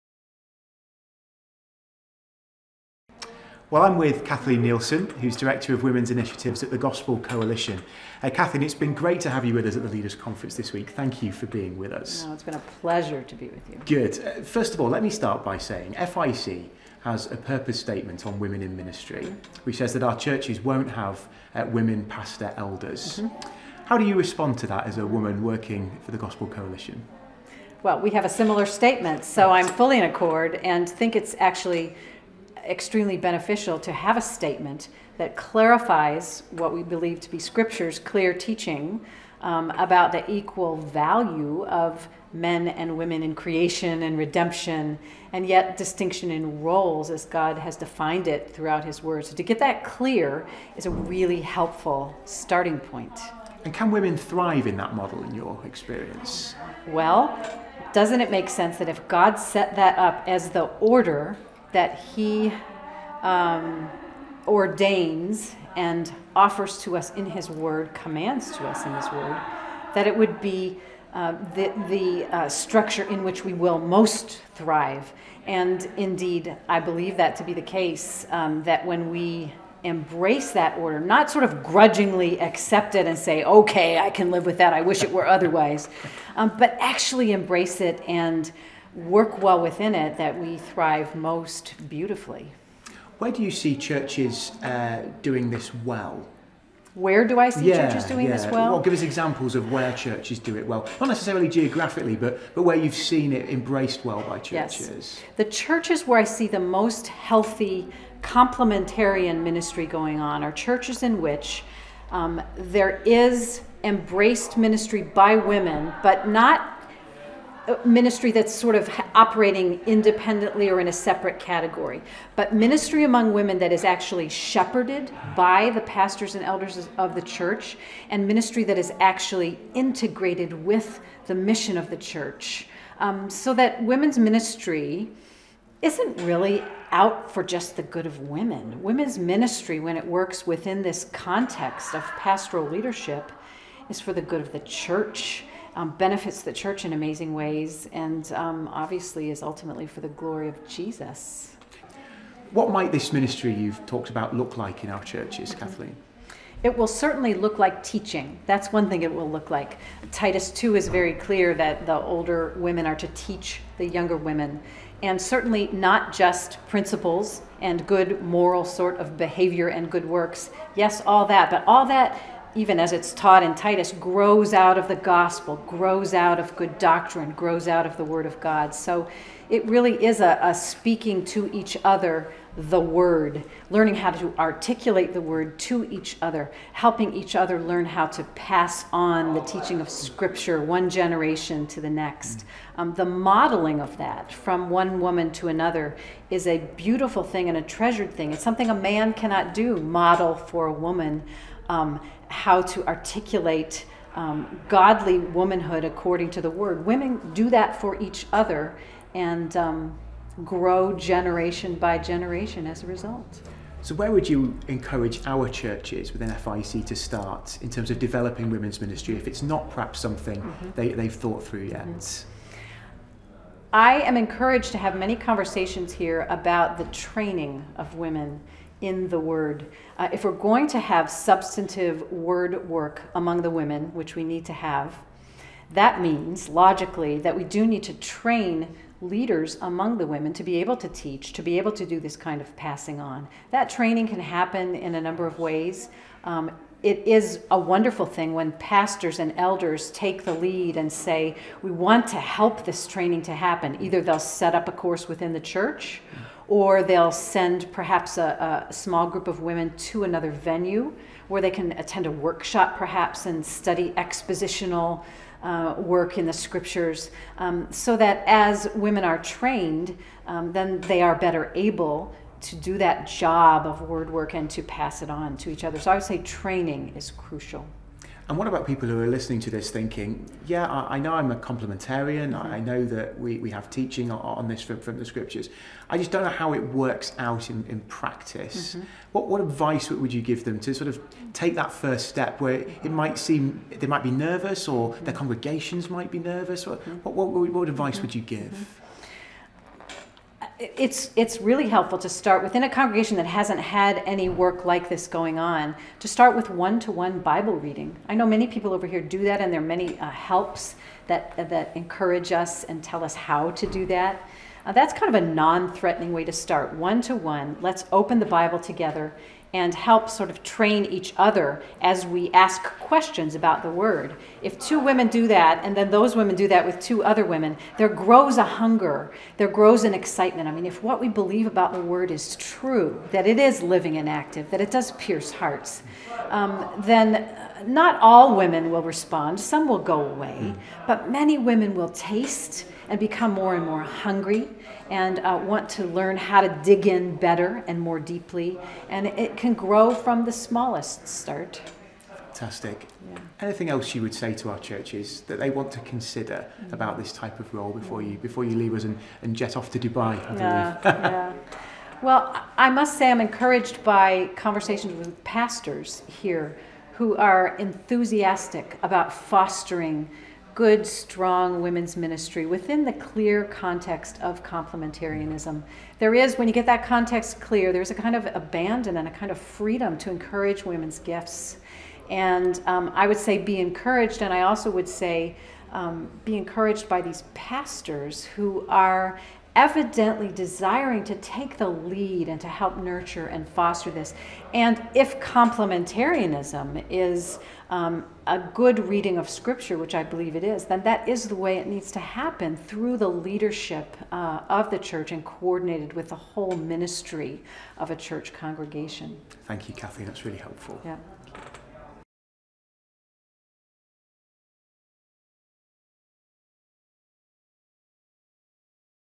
Developing Women's Ministry in Your Church
We were delighted to have Kathleen Nielson at our 2015 Leaders' Conference, so we took the opportunity to ask her some questions about how churches can grow women's ministry.